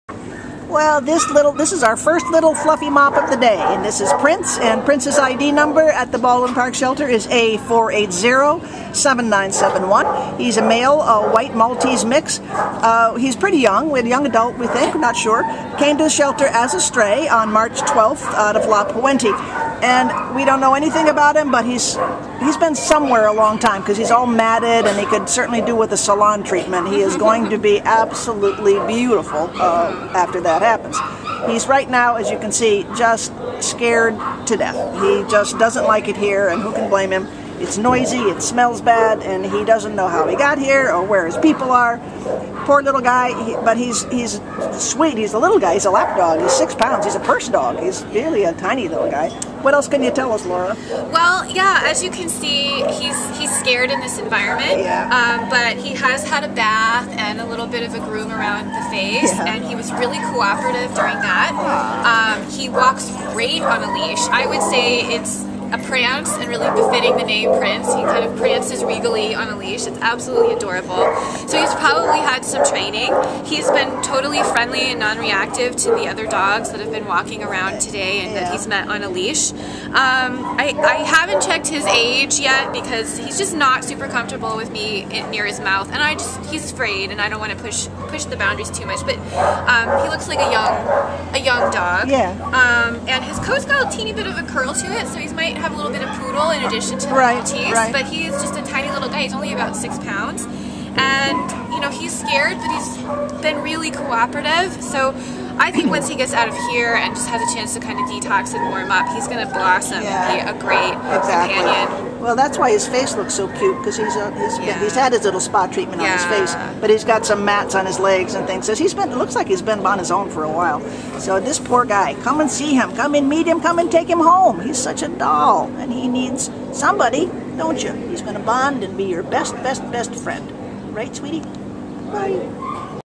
A4807971 Prince
A4807971 Prince is a snuggly 2-year-old white male Maltese and Miniature Poodle mix who was found in La Puente on March 12th and brought to the Baldwin Park Animal Care Center. Weighing 6 lbs, Prince finds it difficult to relax in the shelter environment, but we think his worries will quickly melt away once he is in a home again. Prince walks regally on leash, and was friendly and polite with both large and small dogs he has met while out with our volunteers. With his delightfully soft coat, Prince is a perfect companion for cuddling, and he will be the most handsome boy on the block with regular grooming. Prince will be a charming and loyal companion to an individual or family with kids old enough to respect his small size.